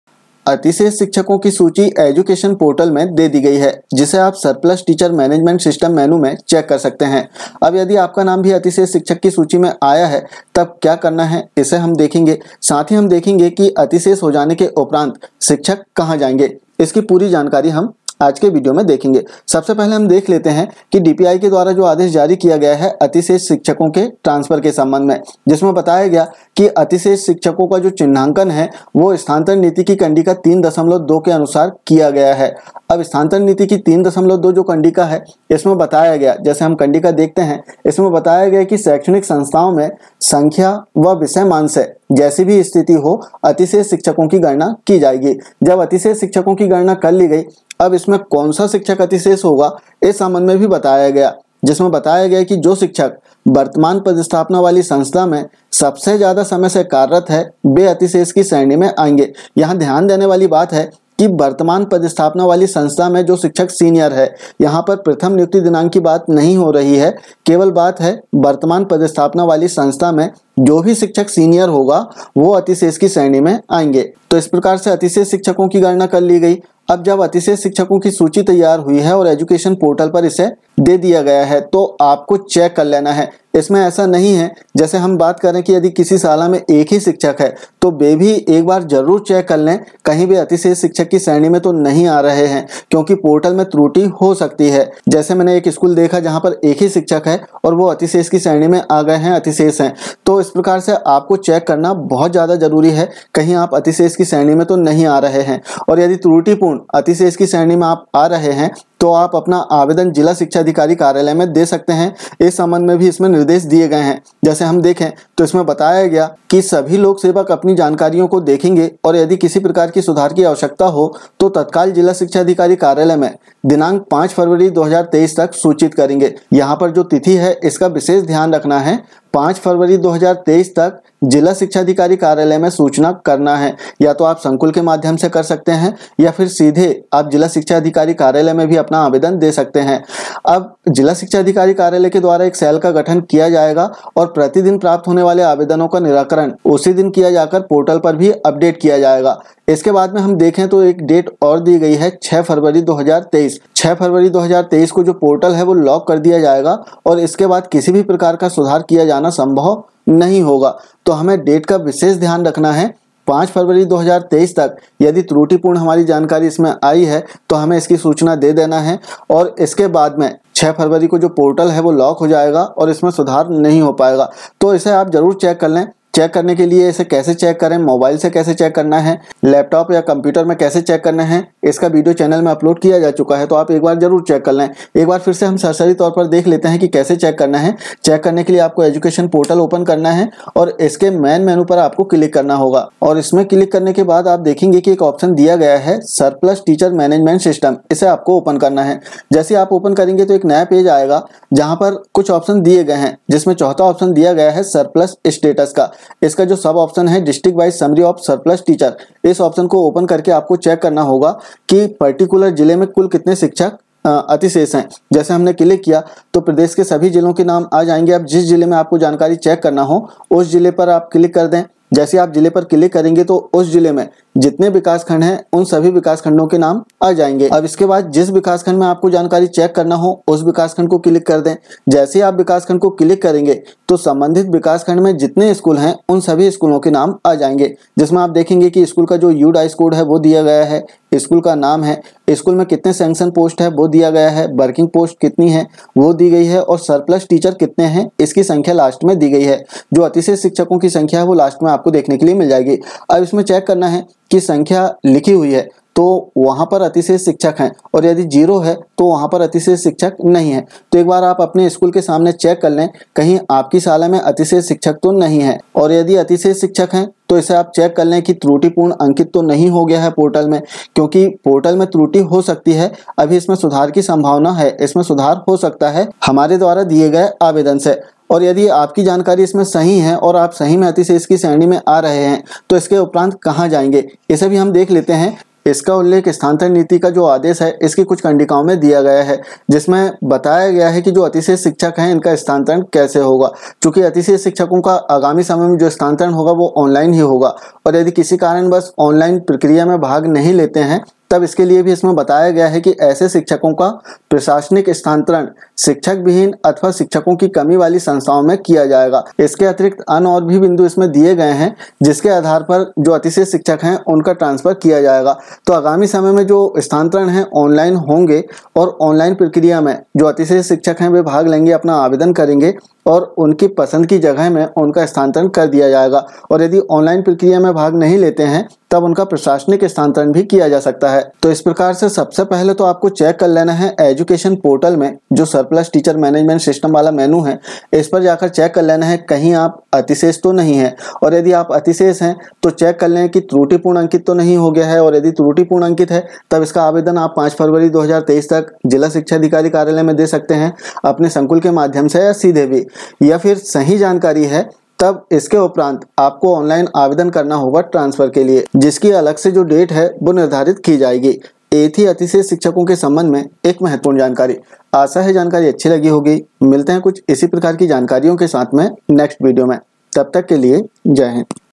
अतिशेष चिन्हांकन के उपरांत क्या करें ? शिक्षक अतिशेष हो गए अब क्या करना है ? कैसे होगा स्थानांतरण ?
अतिशेष चिन्हांकन के उपरांत क्या करें ?
शिक्षक अतिशेष हो गए अब क्या करना है ?
कैसे होगा स्थानांतरण ?